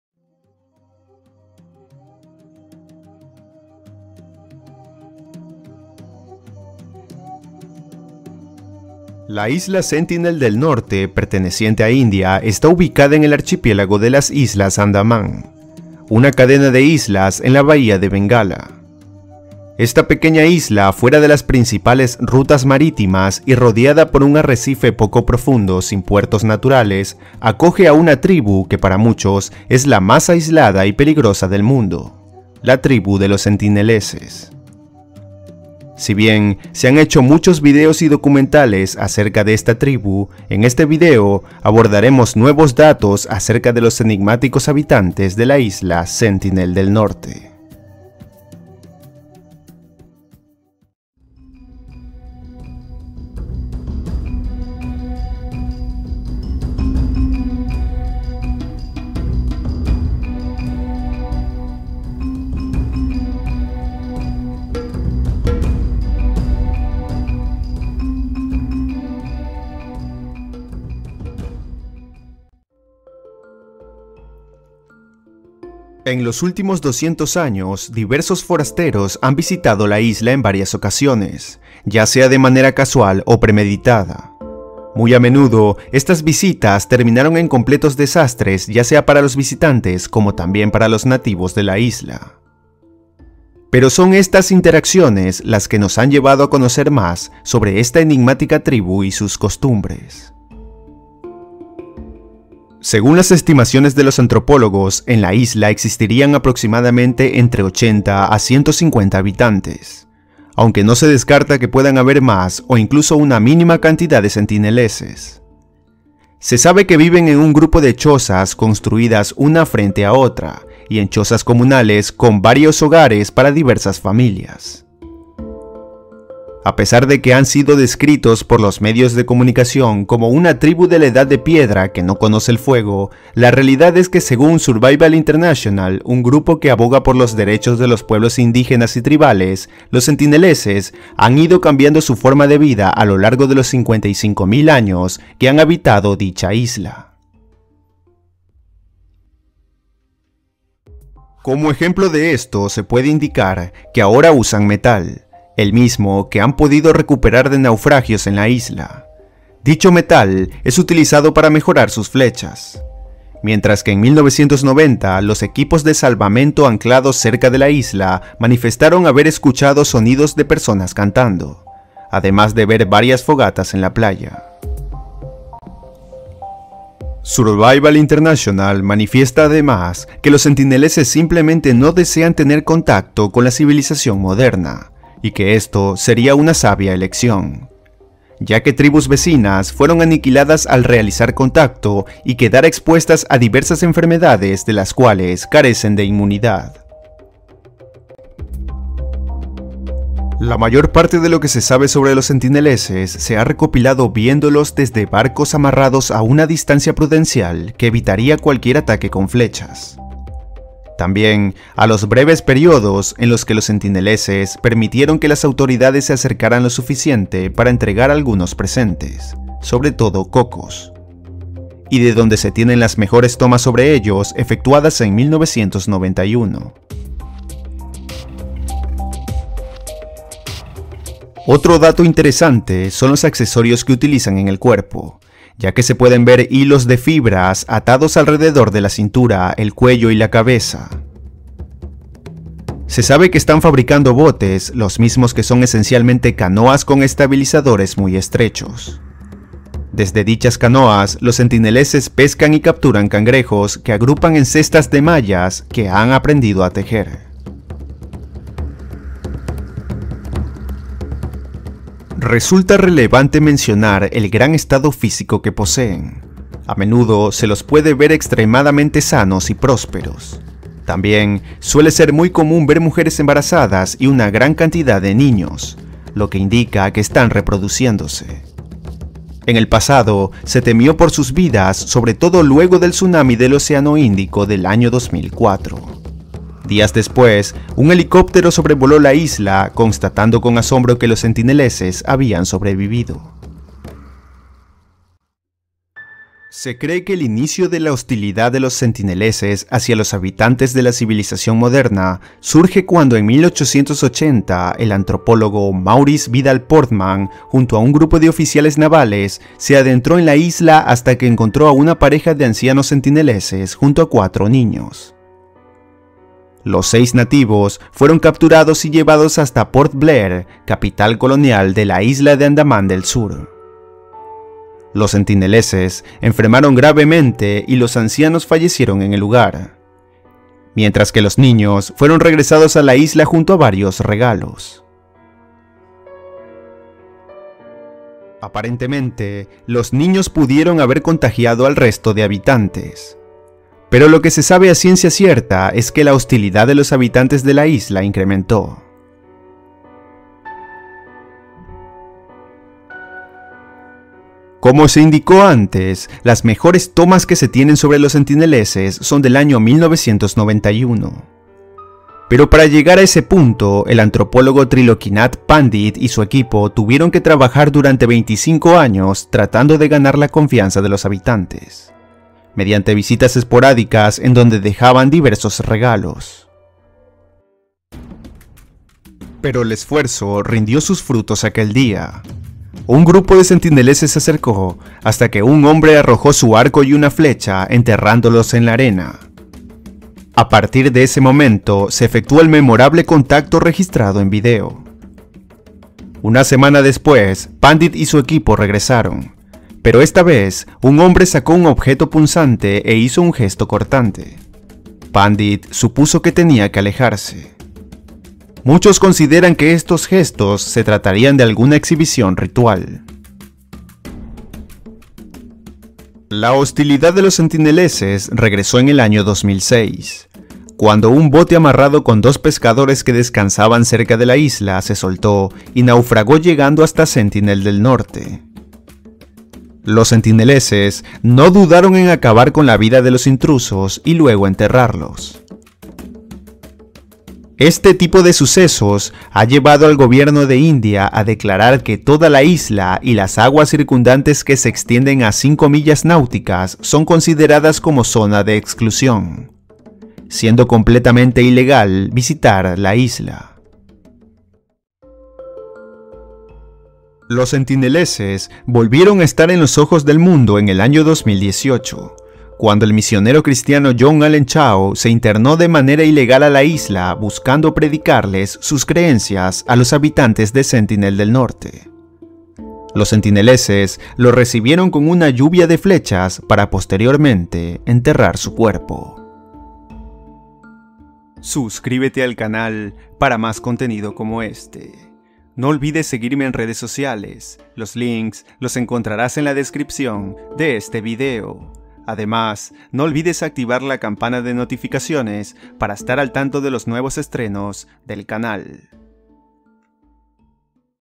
Los Sentineleses, La Tribu más Aislada y Peligrosa del Mundo🔥🌴 (Nuevos Datos)
La isla Sentinel del Norte perteneciente a India, está ubicada en el archipiélago de las islas Andamán, una cadena de islas en la bahía de Bengala. Esta pequeña isla, fuera de las principales rutas marítimas y rodeada por un arrecife poco profundo sin puertos naturales, acoge a una tribu que para muchos es la más aislada y peligrosa del mundo, la tribu de los sentineleses.
En los últimos 200 años, diversos forasteros han visitado la isla en varias ocasiones, ya sea de manera casual o premeditada. Muy a menudo, estas visitas terminaron en completos desastres, ya sea para los visitantes como también para los nativos de la isla. Pero son estas interacciones, las que nos han llevado a conocer más, sobre esta enigmática tribu y sus costumbres.
Si bien, se han hecho muchos vídeos y documentales acerca de esta tribu, en este vídeo abordaremos nuevos datos acerca de los enigmáticos habitantes de la isla Sentinel del norte.

Eso sería de mucha ayuda para este proyecto.

⚠️Nota Aclaratoria:
Este vídeo tiene fines informativos, no pretende generar ningún tipo de ofensa o de polémica.